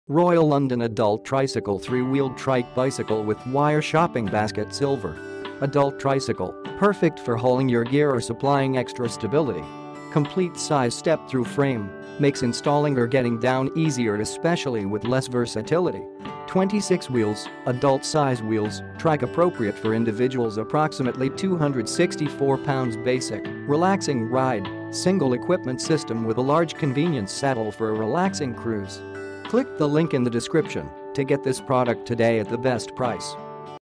Royal London Adult Tricycle 3 Wheeled Trike Bicycles w Wire Shopping Basket Silver
Video Title: Royal London Adult Tricycle 3 Wheeled Trike Bicycles w Wire Shopping Basket Silver

Video File Created Date: Thursday, September 12, 2019 (Video may or may not have been captured on this date, it shows the date the video was last converted.)

GROWN-UP TRICYCLE - Perfect for hauling your equipment or offering additional security
COMPLETE DIMENSIONS STEP WITH FRAMEWORK - Makes mounting or getting down simpler especially with much less versatility
26" WHEELS - Adult dimension wheels, trike ideal for users up to 264lbs
SIMPLE, RELAXING RIDE - Solitary equipment system with a broad convenience saddle for a relaxing cruise ship